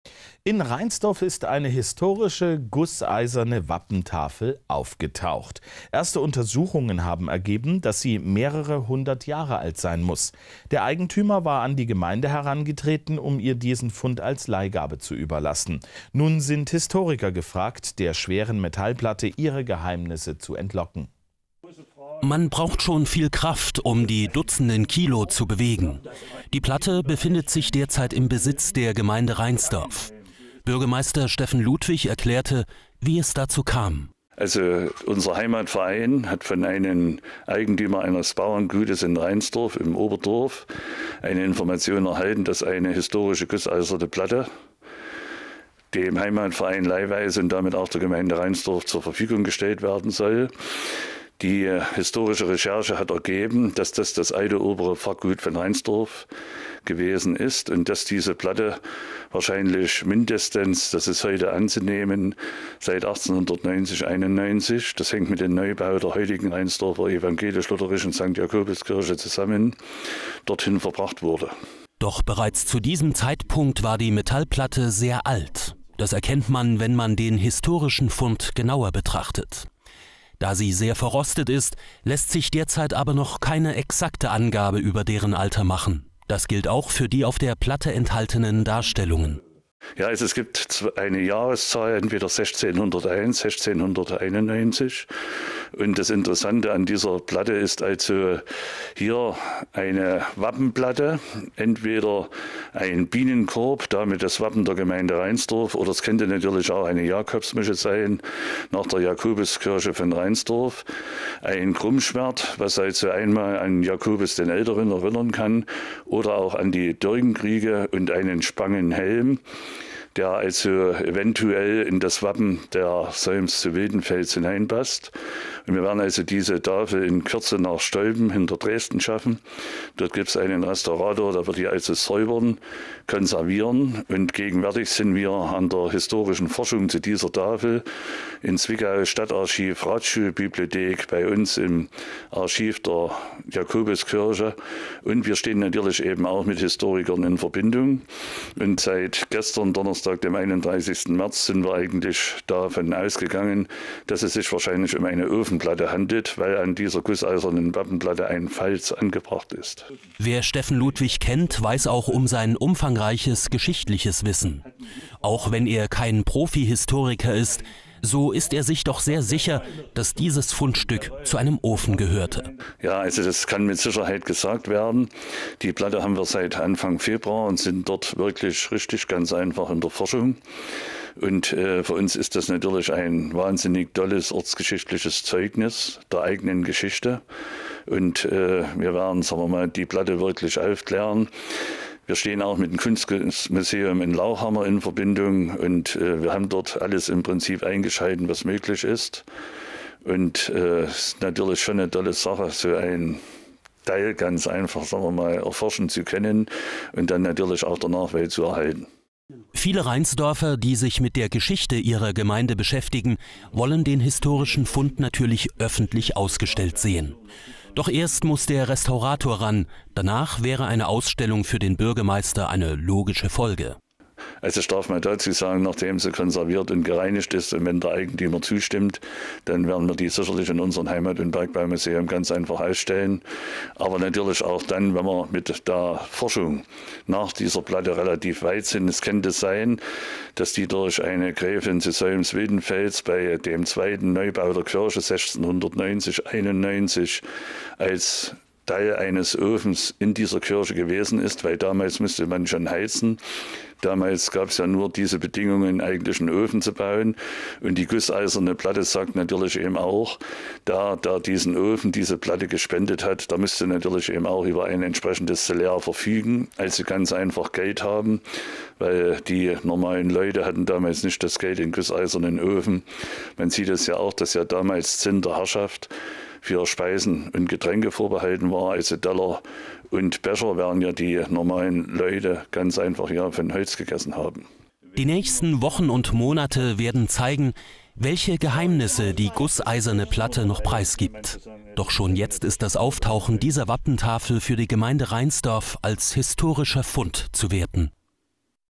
Historischer Fund in Reinsdorf
In Reinsdorf ist eine historische gusseiserne Wappentafel aufgetaucht. Erste Untersuchungen haben ergeben, dass sie mehrere hundert Jahre alt sein muss. Der Eigentümer war an die Gemeinde herangetreten, um ihr diesen Fund als Leihgabe zu überlassen. Nun sind Historiker gefragt, der schweren Metallplatte ihre Geheimnisse zu entlocken.